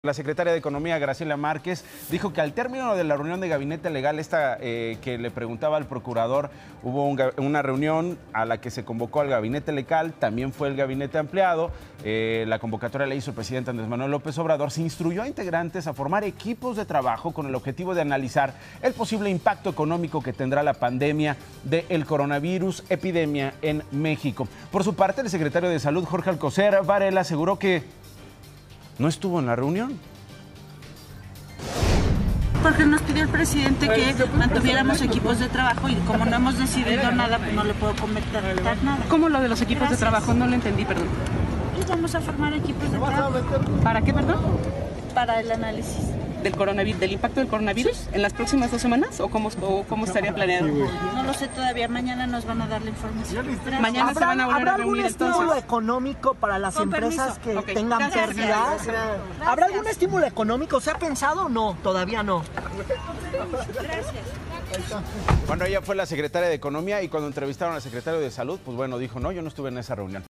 Grupos de trabajo para analizar impacto económico por Covid-19 en México | De Pisa y Corre
La secretaria de Economía, Graciela Márquez Colín, afirma que se harán grupos de trabajo para analizar el impacto económico del coronavirus COVID19 en México.